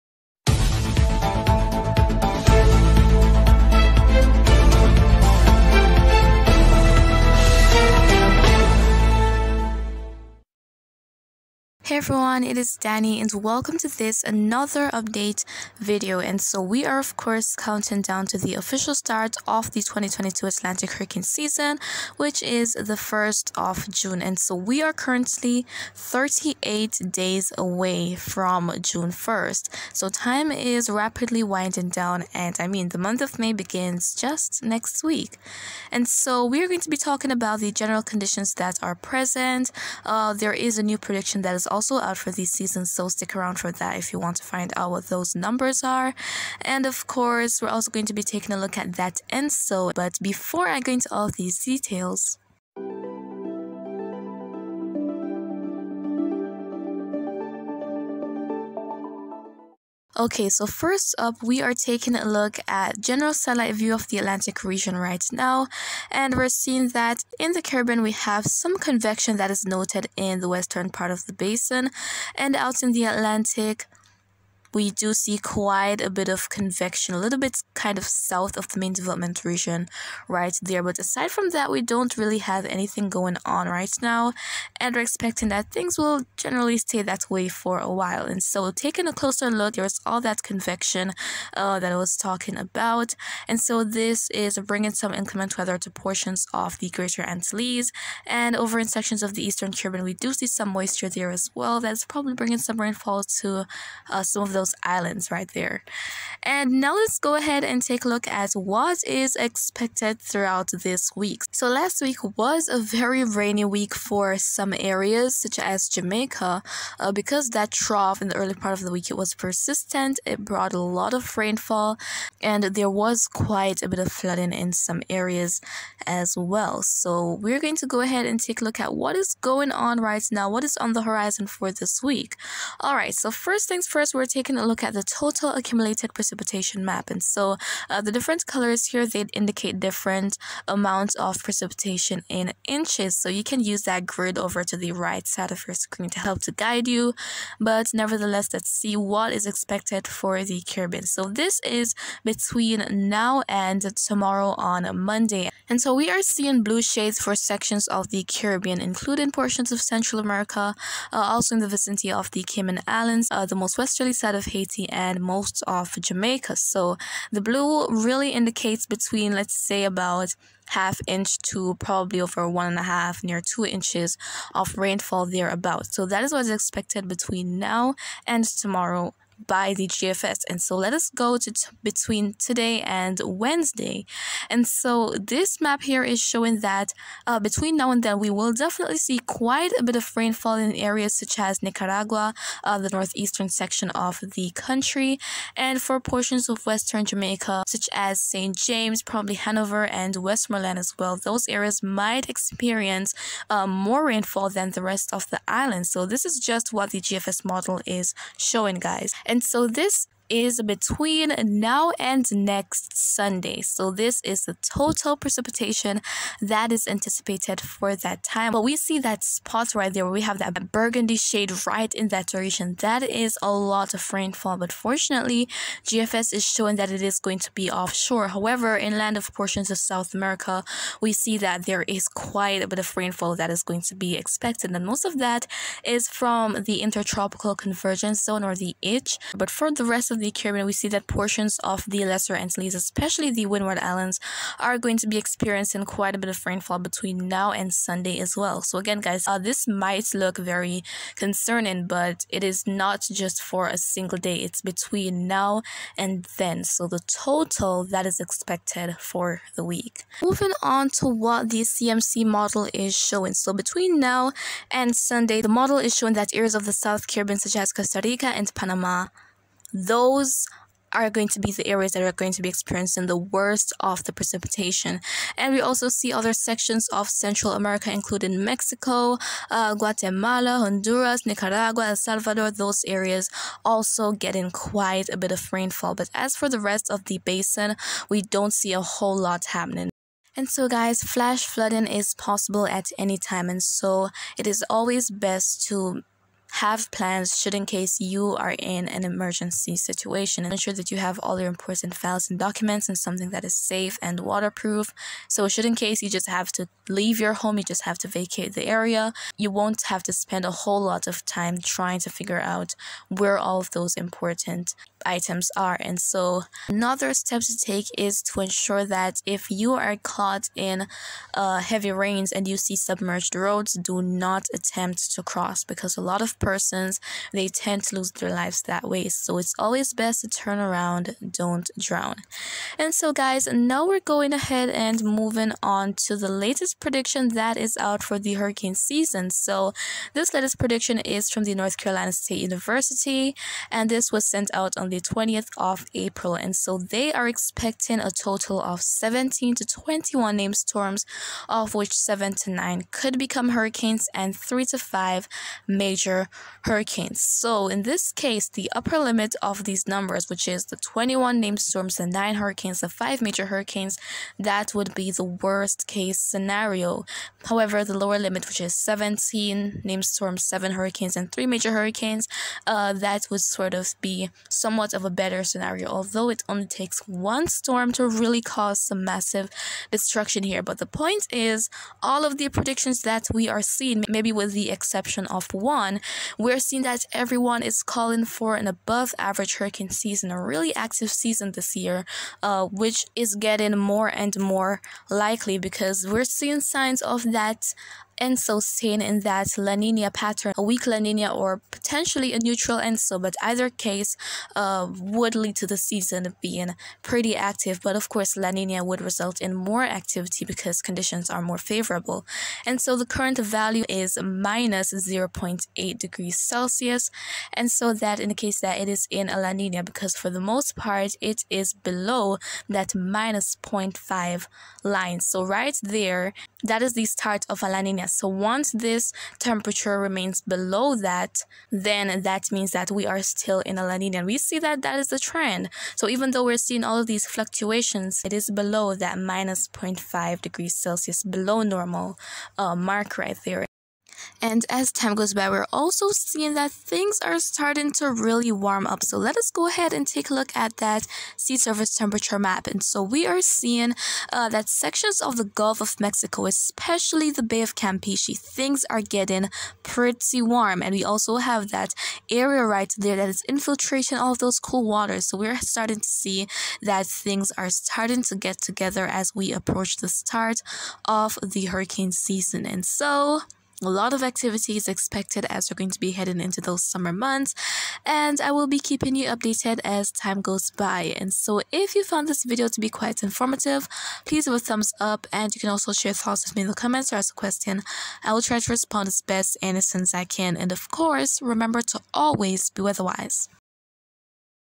2022 Atlantic Hurricane Season countdown (38 days) • Caribbean Weather Update
The Atlantic hurricane season commences in 38 days and this video covers all you need to know, including a new prediction that was released.

~weather.gurl.danii